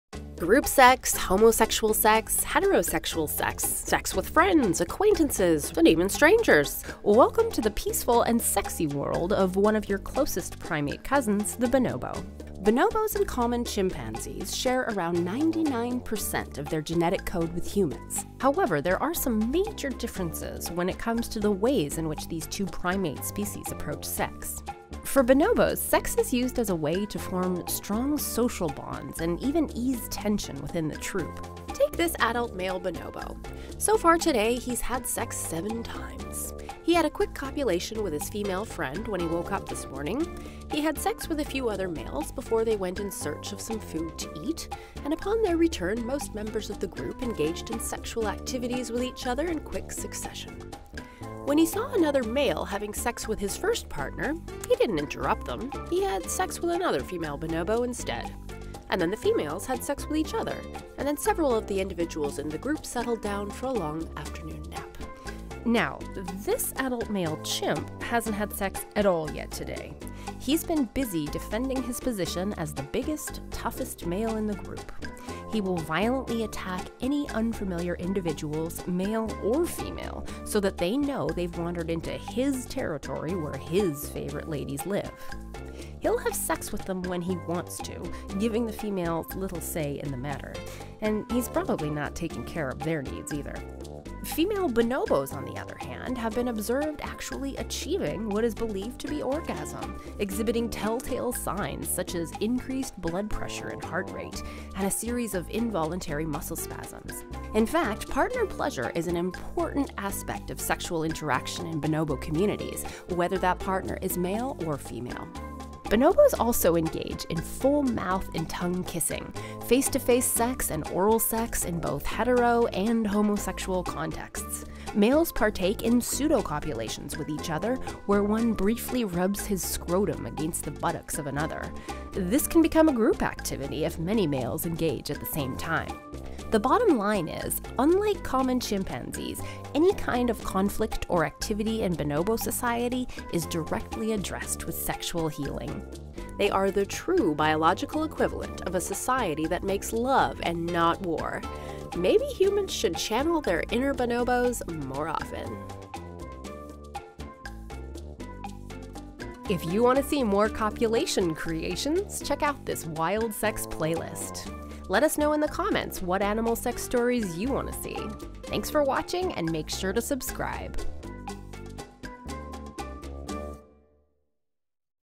Making Love Not War: The Wild Sexual World of Bonobos
When it comes to sex, Bonobos do it all. There is, however, biological reasons behind all these sexual encounters. So why exactly do bonobos randevú round the clock?

“Bonobos engage in sex in virtually every partner combination (although such contact among close family members may be suppressed). And sexual interactions occur more often among bonobos than among other primates.”

“Sexual activity — in many creative forms — plays a large role in bonobo society. Sexual contacts occur often, in virtually all partner combinations and in a slew of different positions.”

“Bonobo hypersexuality appears closely linked to the evolution of female-female alliances.”

Check Out Wild Sex on Facebook!